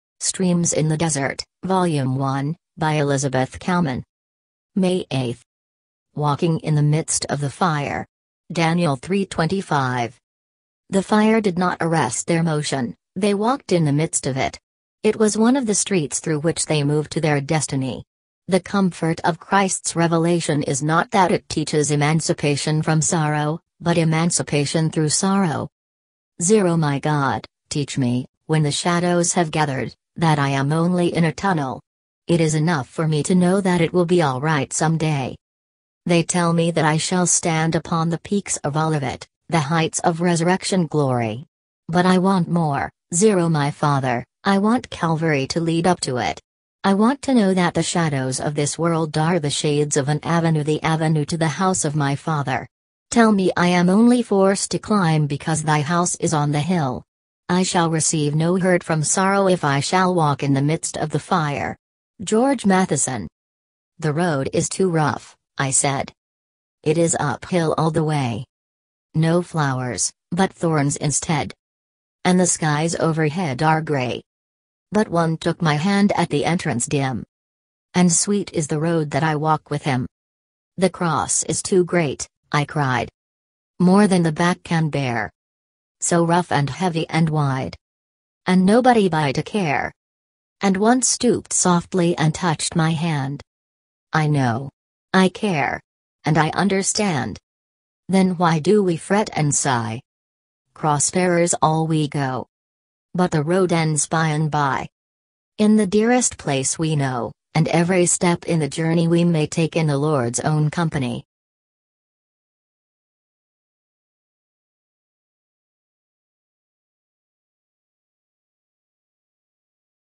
Streams in the Desert - Daily Devotion - Vol 1 - May 08
Streams in the Desert is a powerful collection of meditations, Christian writings, and Scripture, and remains one of the most beloved devotionals of all time.
Charles Cowman is a widely well-known and popular daily Bible devotion. Streams in the Desert offers wisdom and insight for applying Biblical truths to the ups and downs of everyday life.